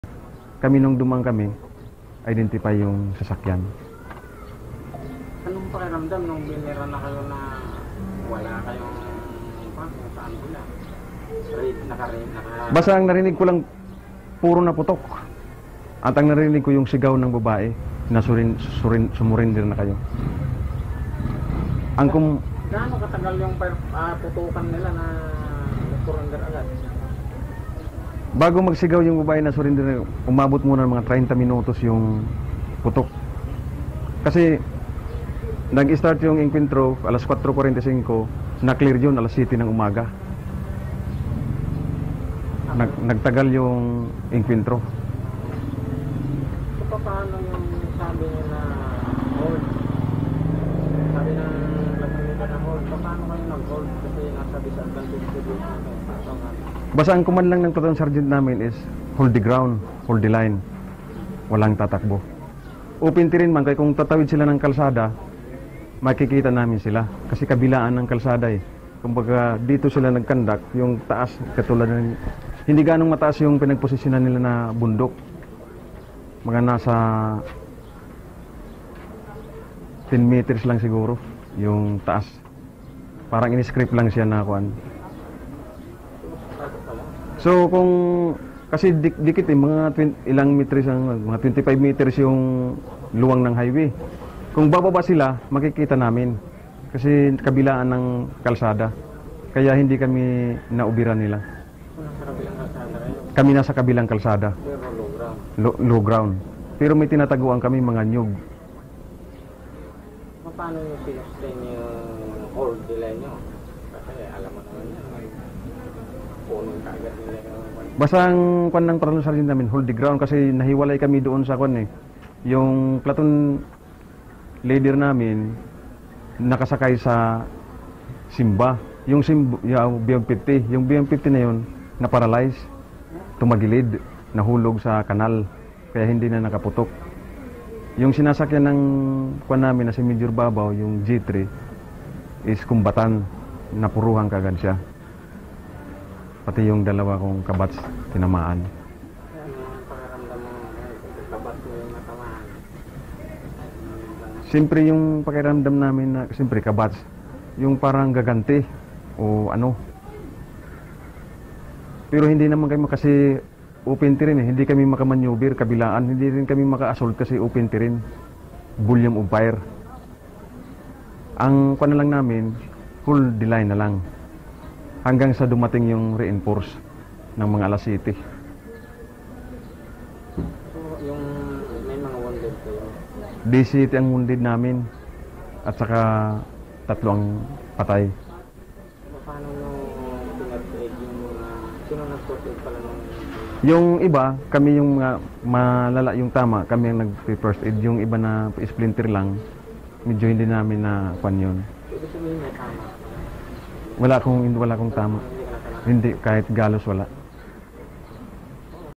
Na ambush kami pero nag hold the line lahat
Ang ma ambush ang iniiwasan ng bawat sundalo. Masyado kasi surprise at planado ito na kung magaling ang mag ambush ay madami ang madali. Sina SSg Ruben Tiston ay na ambush ng NPA sa Samar ng madaling araw. Narinig na lang niya puro putukan at ang sigaw ng babaeng NPA na mag surrender na sila. Pero nag hold the line ang mga sundalo kahit dehado ang kanilang posisyon. Matibay kasi ang combat leadership ng kanilang platoon sergeant. Matindi din ang combat actions ng mga kasama nila at di bumigay.